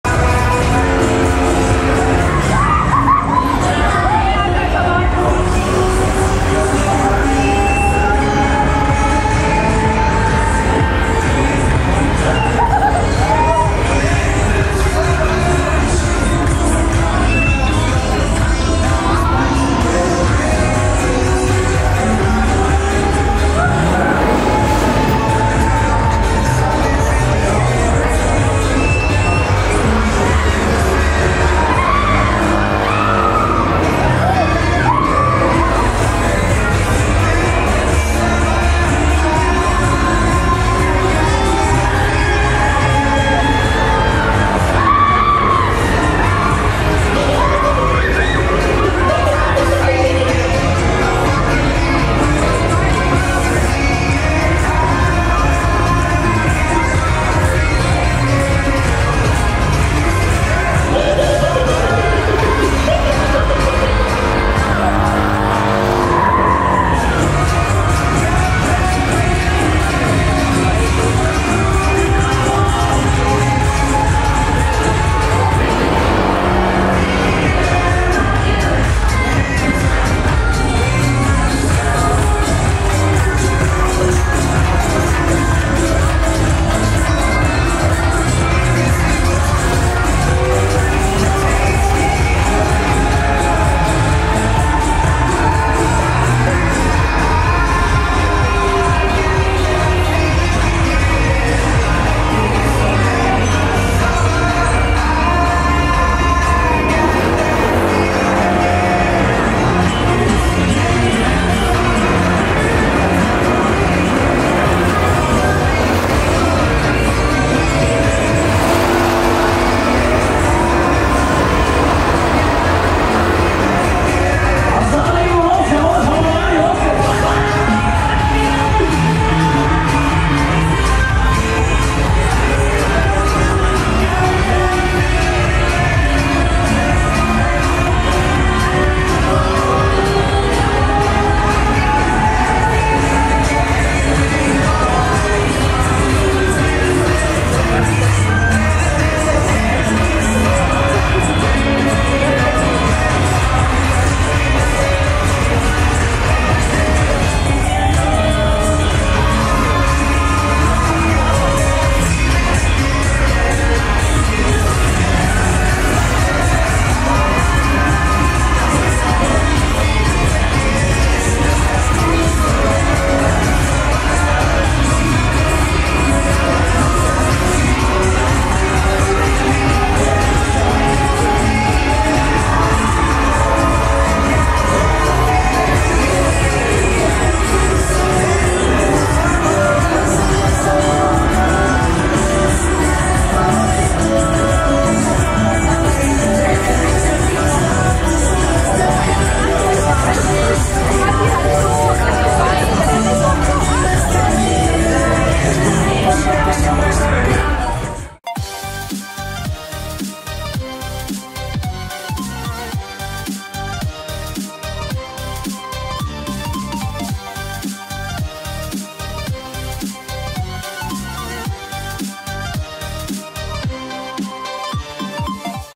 Black Pearl [Frank] | Frühjahrsmesse Magdeburg 2024 | OffRide
Black Pearl / Pirates of the Caribbean - Frühjahrsmesse Magdeburg 2024 OffRide

Statistiken:
Veranstaltung : Frühjahrsmesse Magdeburg
Hersteller : Husek
Typ : Piratenschaukel
Höhe : 15 Meter
Schausteller : Frank

Stats :
Fun Fair : Frühjahrsmesse Magdeburg
Manufacturer : Husek
Type : Pirate Swing
Height : 49 ft [15 m]
Showman : Frank